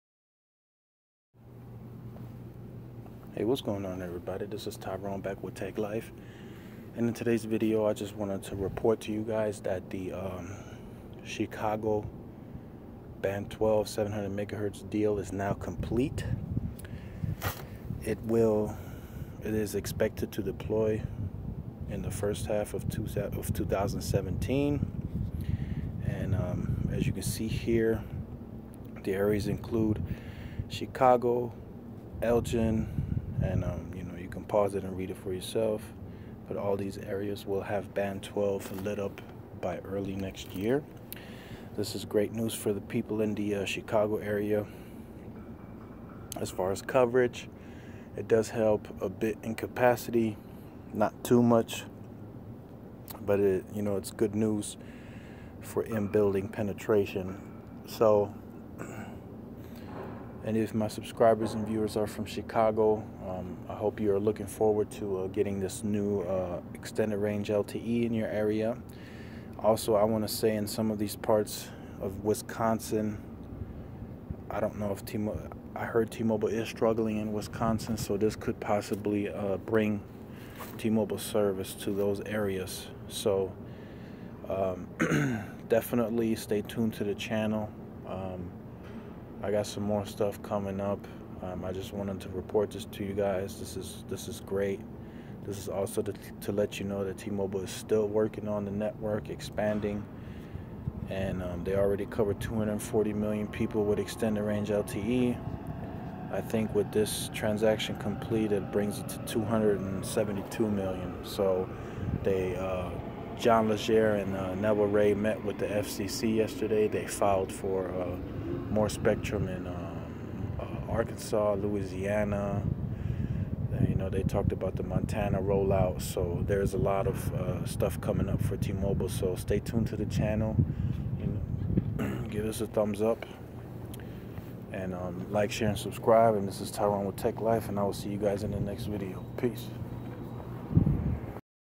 T-Mobile LTE advanced (band 12 700 mhz finally coming to Chicago)
In today's video i wanted to report to you guys the 700 mhz (band 12 ) deal is done in Chicago. Band 12 will be deployed in the 1st half of 2017.